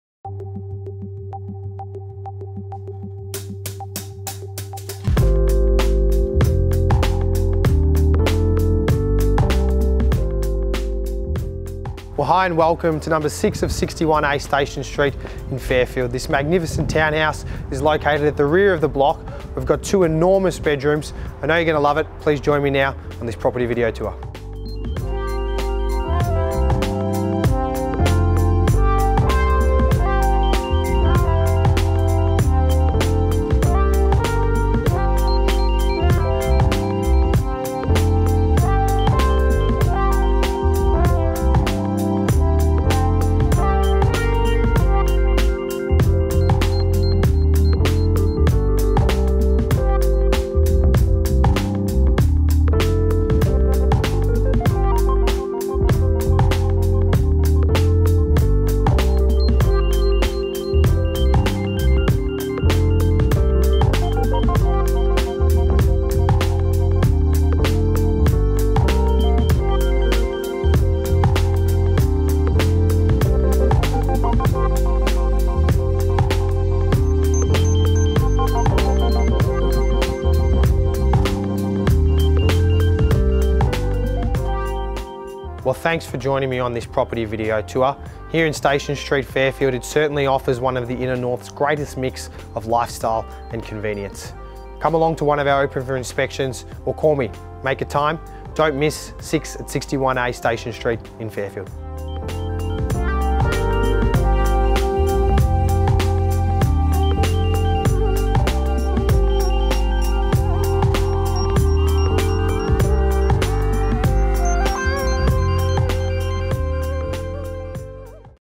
6/61A Station Street, Fairfield For Sale by Kristian Lunardi of Nelson Alexander
or call Kristian Lunardi on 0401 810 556

Set well back from the street in a boutique block of six, this two-level townhouse presents a compelling lifestyle opportunity with its spacious design, sublime surrounds and instant access to Fairfield Station and village cafes. Immaculate throughout, the home's inviting interior boasts two extra-large upstairs bedrooms (both with robes) and central bathroom. Downstairs: spacious lounge/dining leading through to the separate meals area and quality granite-bench kitchen with Blanco cooking appliances and dishwasher. Add to this split-system heating and cooling (both bedrooms and lounge), polished hardwood floors, laundry and powder room (second WC), plus a private north-facing courtyard and undercover parking space on title. Also close to shops and supermarkets, Fairfield Primary School, easy access to riverside trails and parkland. Low-maintenance perfection!